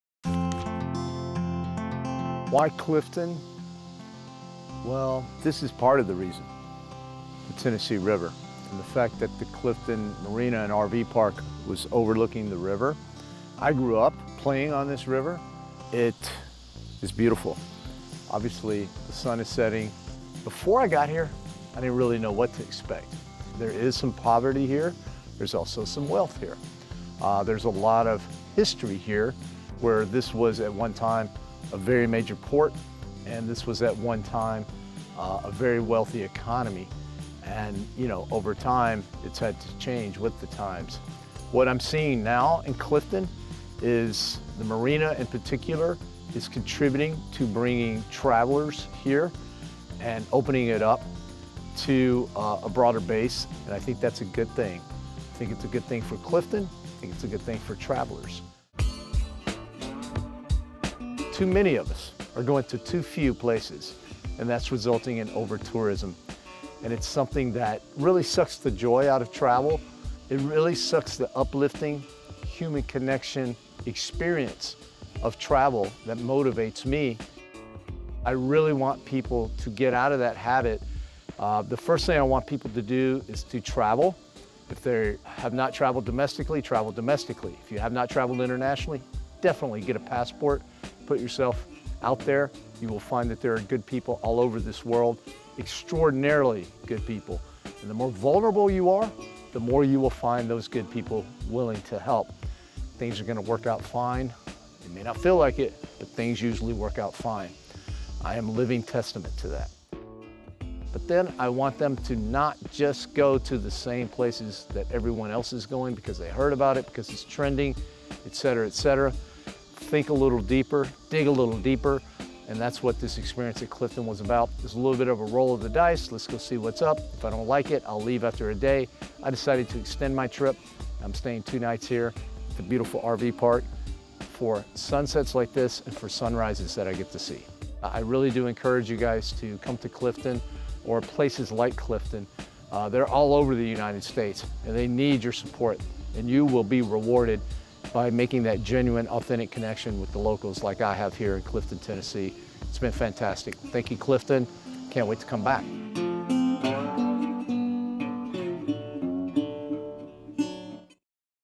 903 Webcut - Rob Talks about visiting Clifton, TN & small-town USA
Rob discusses visiting Clifton, Tennessee, and what he likes about visiting small-town America, especially when on an RV Road Trip.